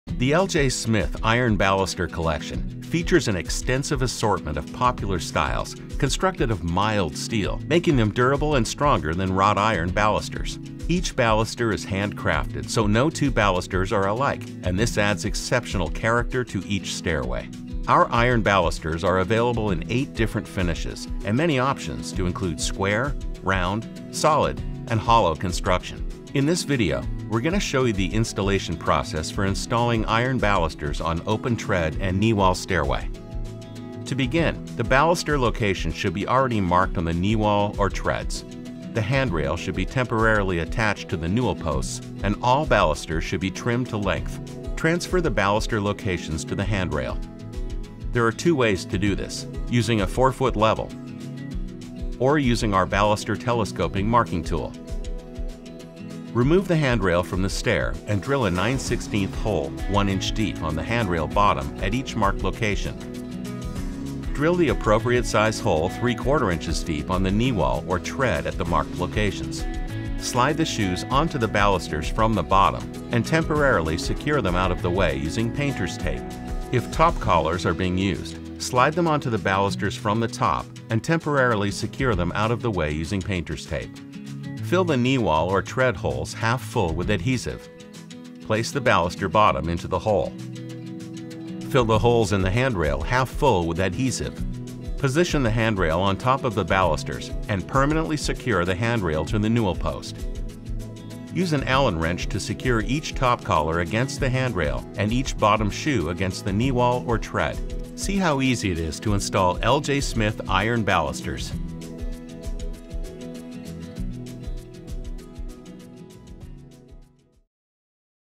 Iron Baluster Installation Guide
Learn how to install our Iron Balusters with this installation tutorial.